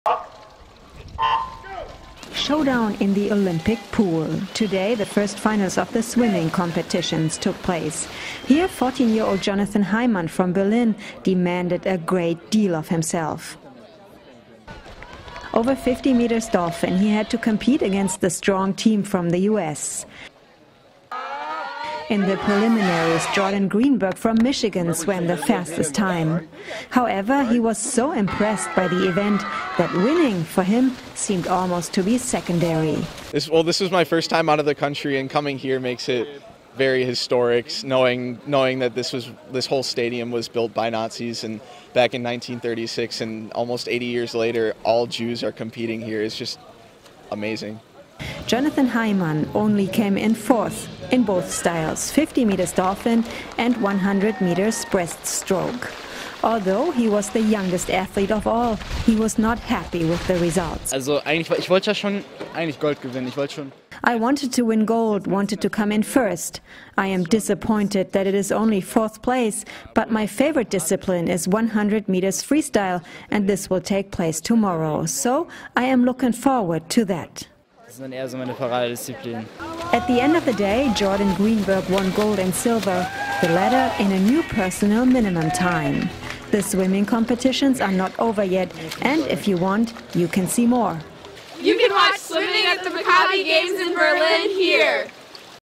European Maccabi Games 2015 | Swimming – Day 3
Showdown at the Maccabi swimming competition. A strong US team challenges various nations. The historic scenery gives the event a special setting.